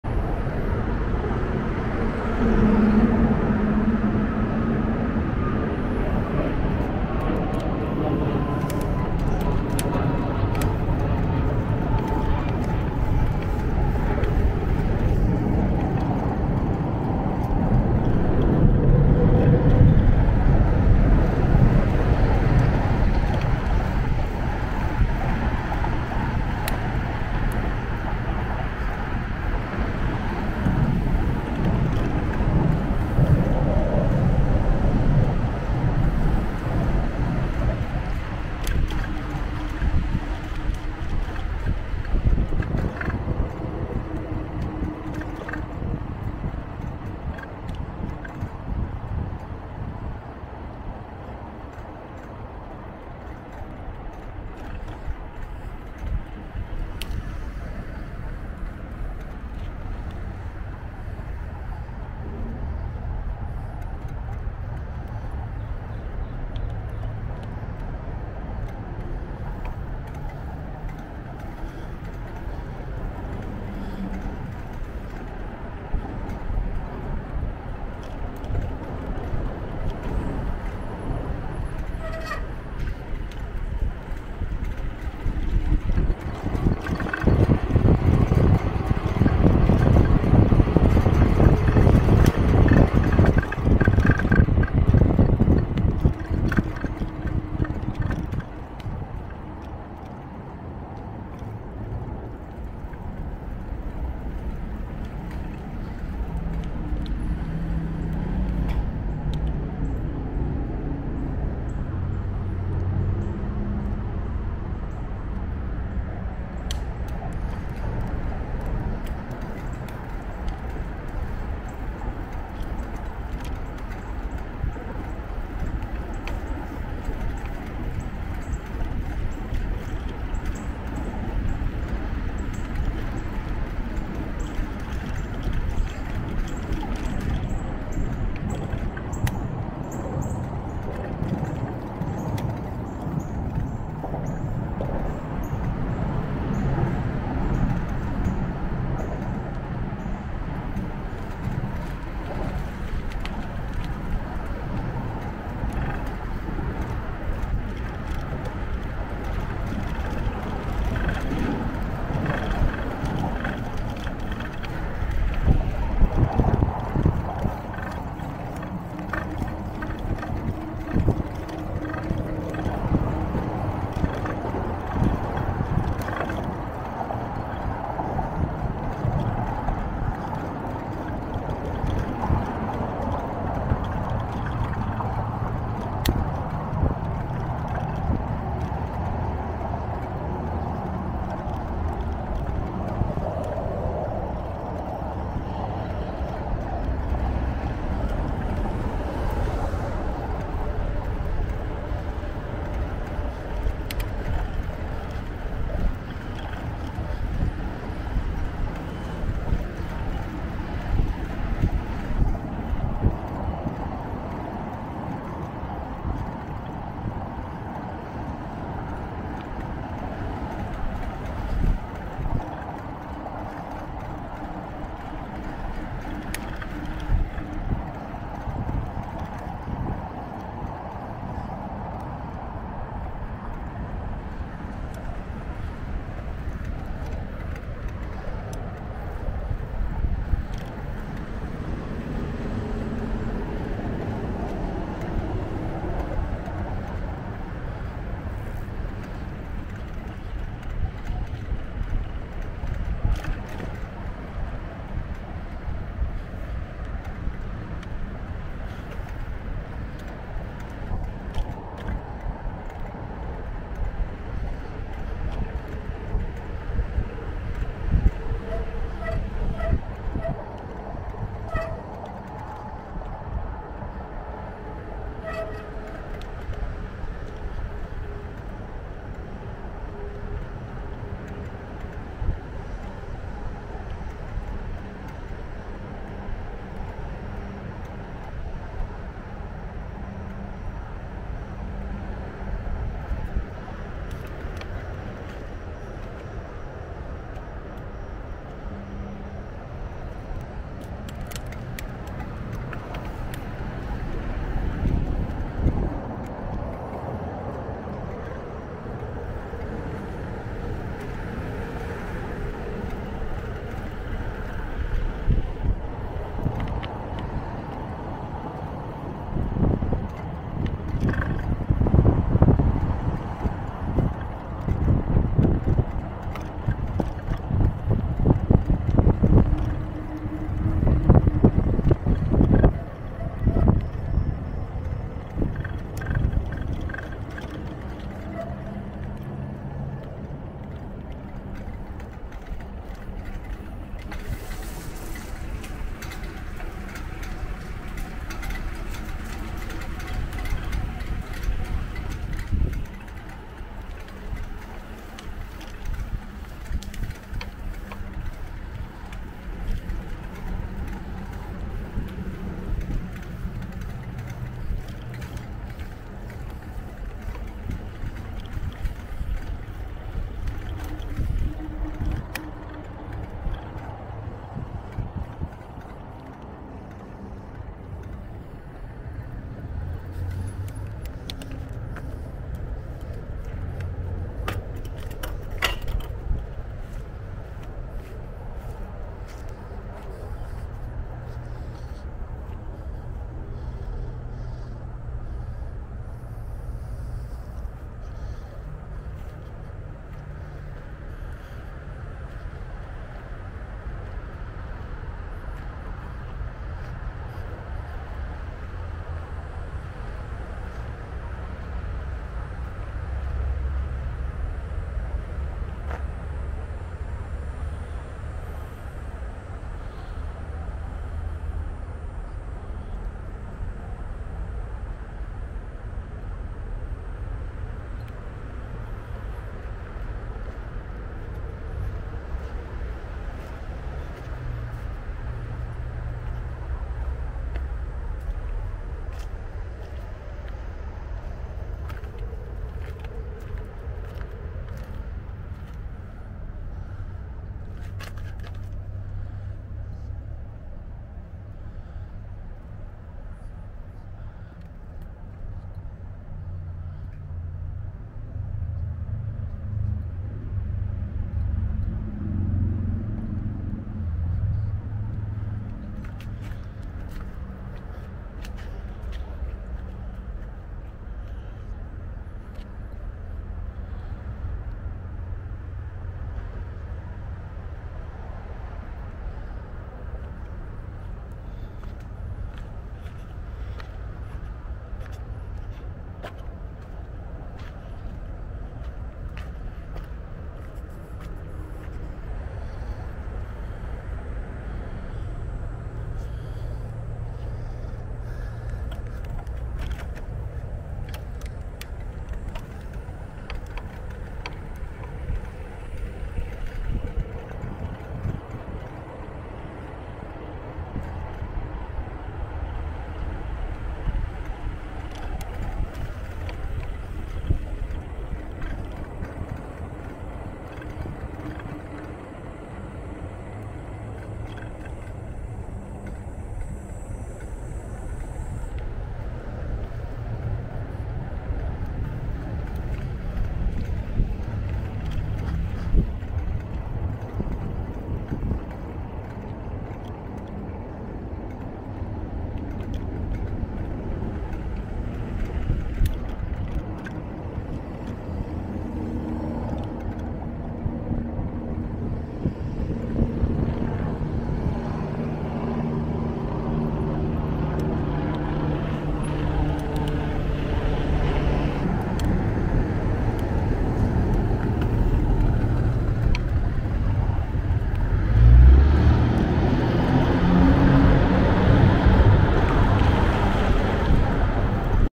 Johnson Creek Trail by bike, from Ladybird Lake to Enfield
Johnson Creek Trail by bike, from Ladybird Lake to Enfield Rd., Austin TX